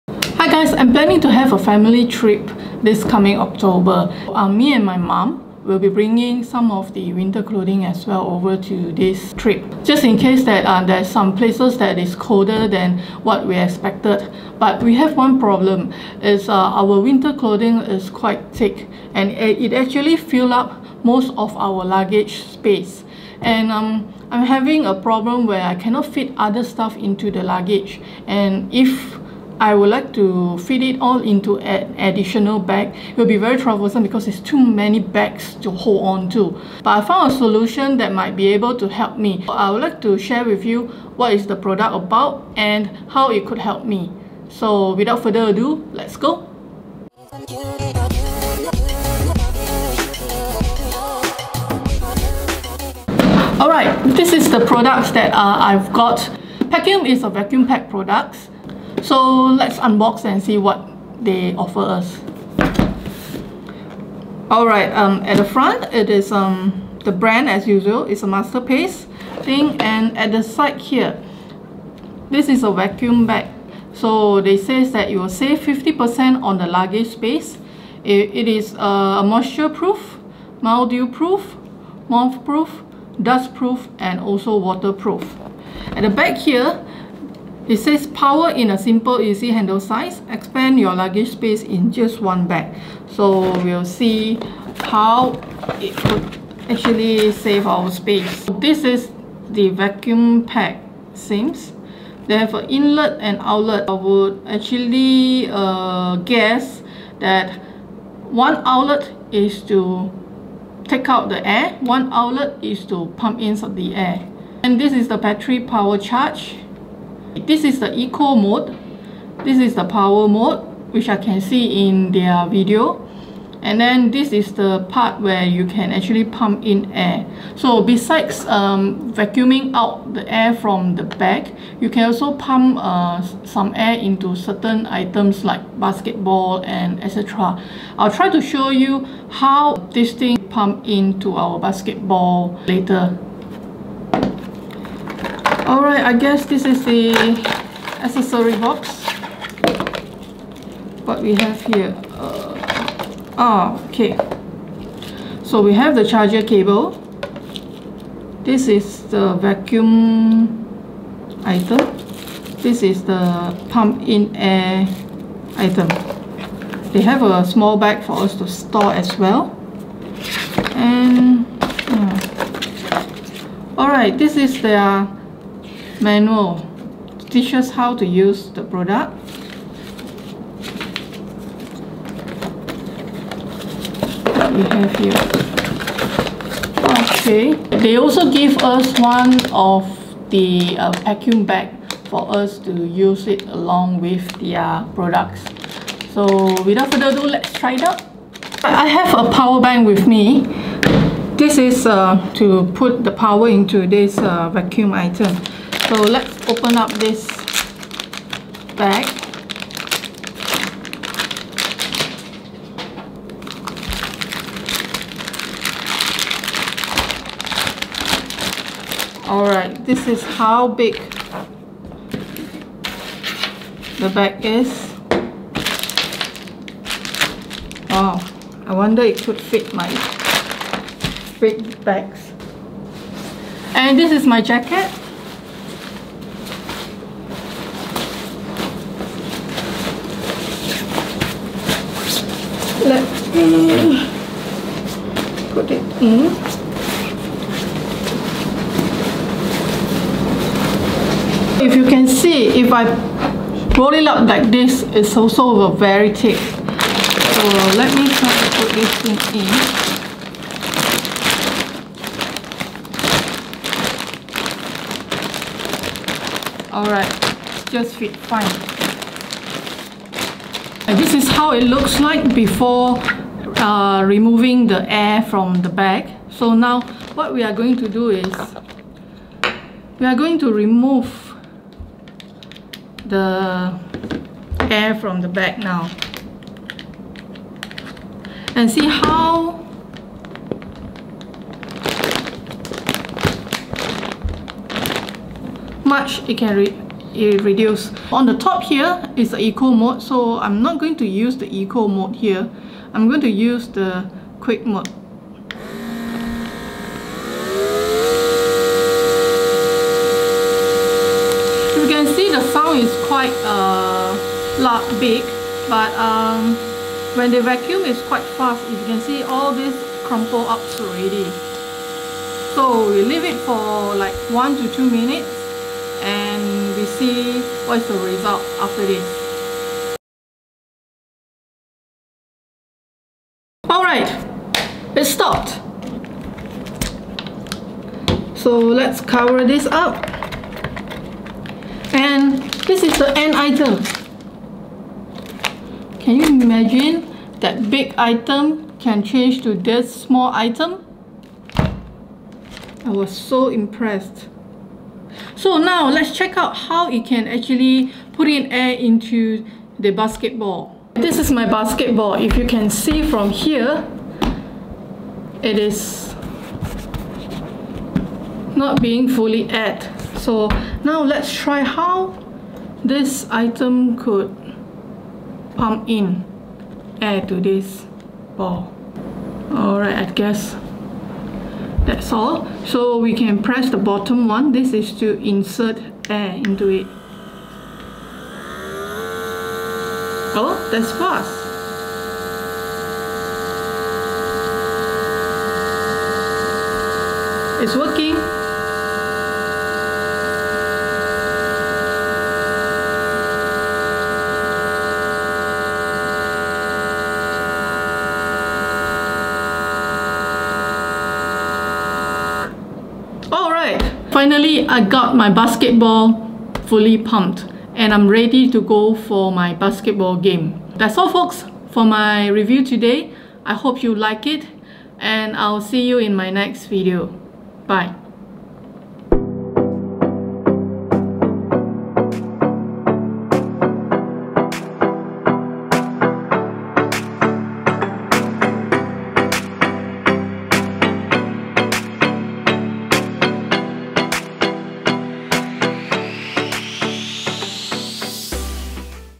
Saved my luggage space in just 2 mins [Review Vlog 12] 仅需2分钟就可以节省行李空间
I will be travelling soon and I realised that my luggage bag do not have enough space for my stuff. One of my friend has recommended me a travel friendly vacum product from Indiesgogo called Pacum.

I have created this video to share with everyone on my unboxing of Pacum and also my impression on it.

For those who are interested to purchase one of this, you may check out Indiegogo

My Tools:

Camera Lens: Sony Vario-Tessar T* FE 16-35mm f/4 ZA OSS